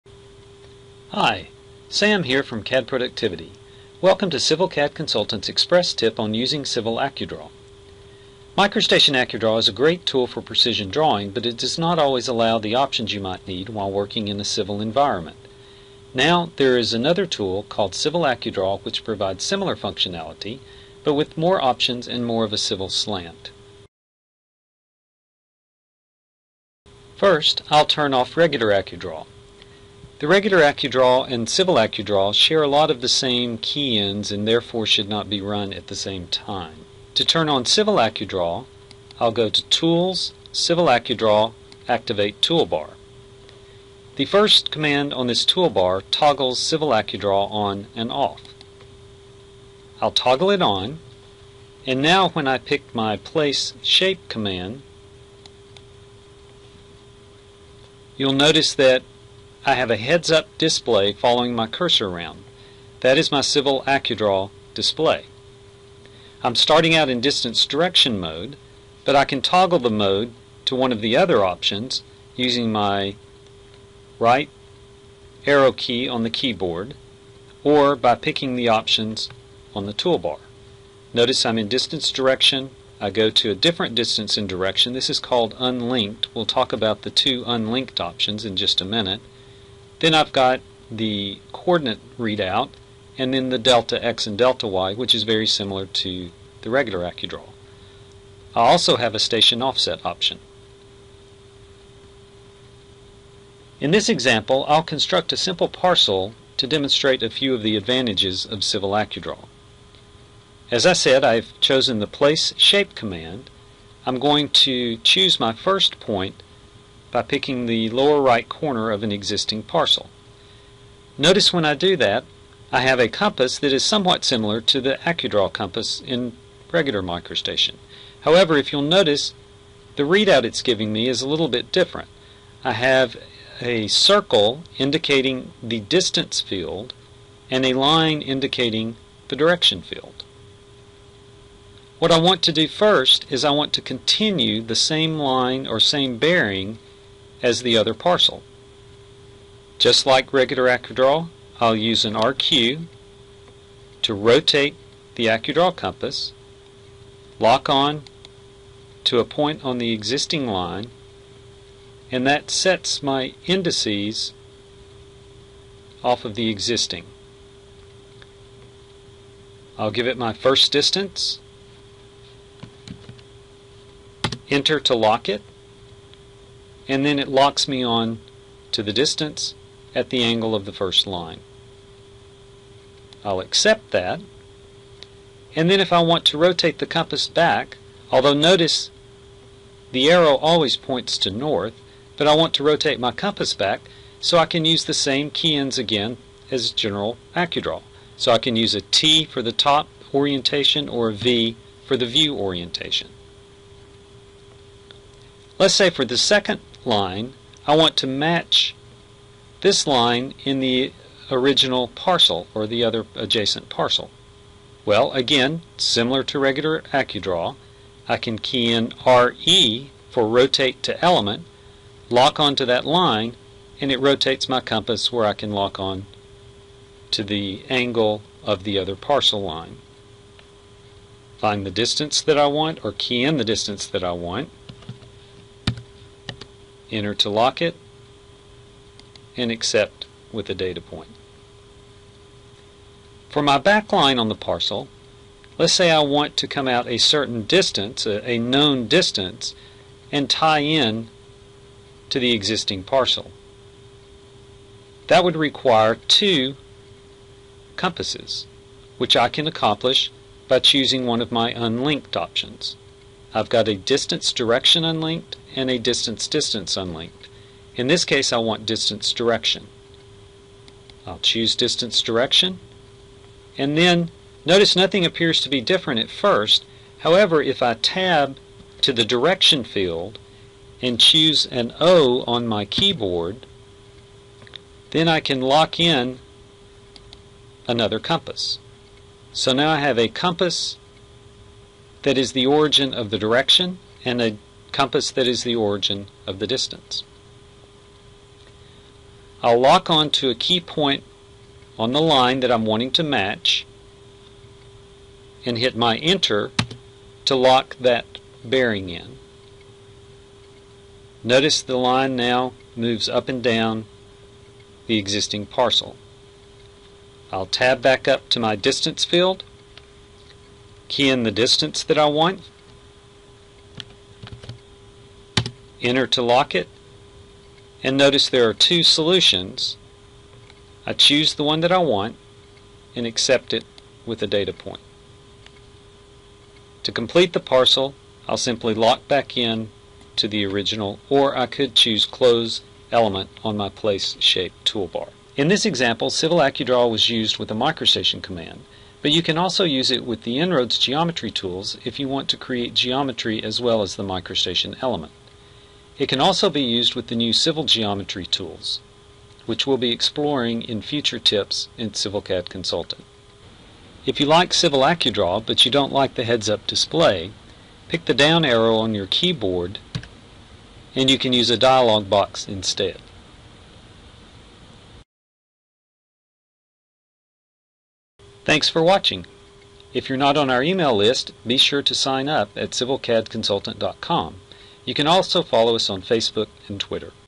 MicroStation Tip: Civil AccuDraw
Civil AccuDraw adds more functionality to the standard AccuDraw making it more useful in the civil environment, and helping you to draw more efficiently in MicroStation. for more InRoads and MicroStation tips. for InRoads training guides and MicroStation training guides.